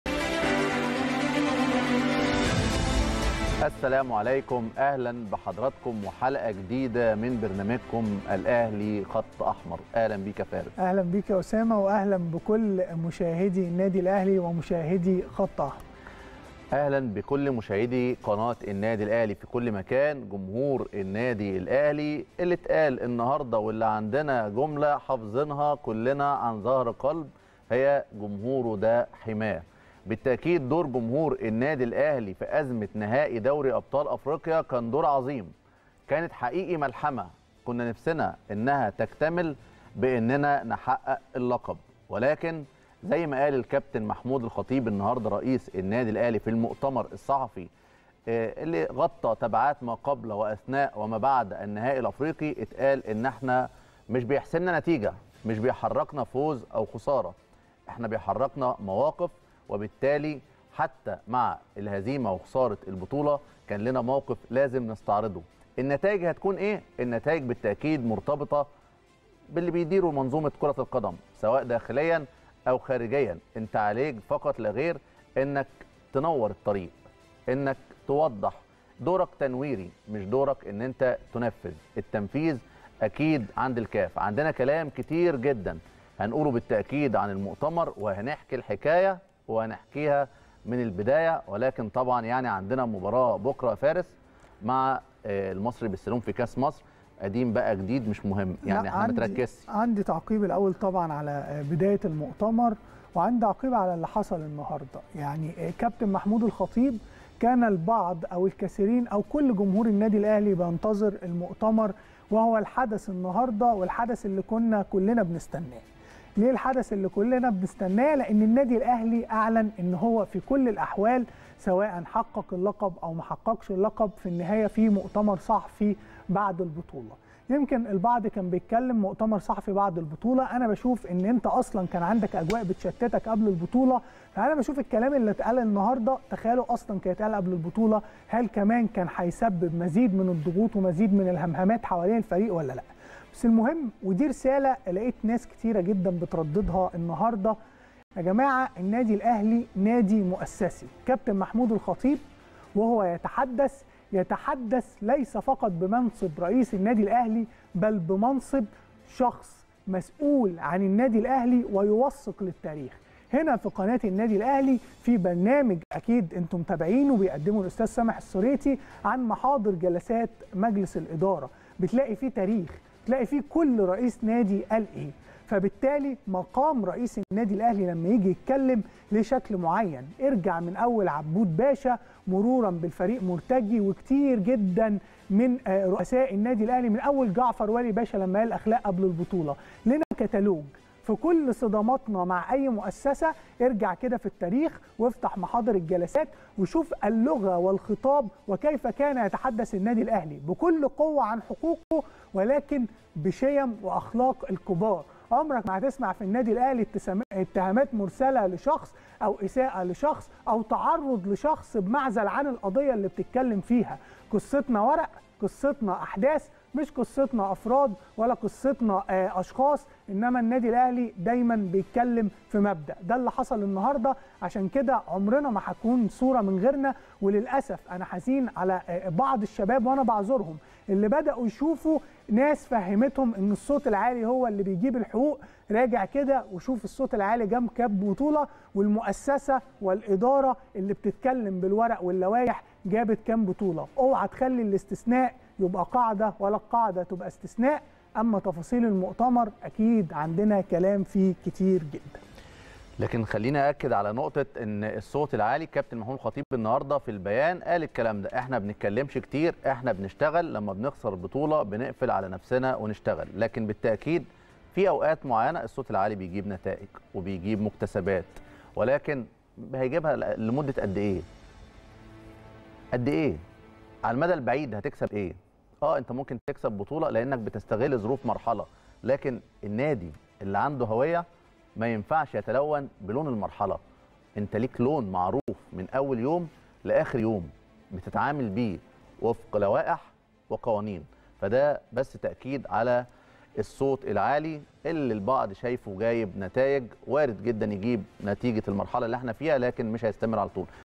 أحمد اسامة ومحمد فارس: النادي الأهلي لا يطالب سوى بالعدالة.. وعمرنا ما هنكون صورة من غيرنا
أحمد اسامة ومحمد فارس: النادي الأهلي لا يطالب سوى بالعدالة.. وعمرنا ما هنكون صورة من غيرنا | الأهلي خط أحمر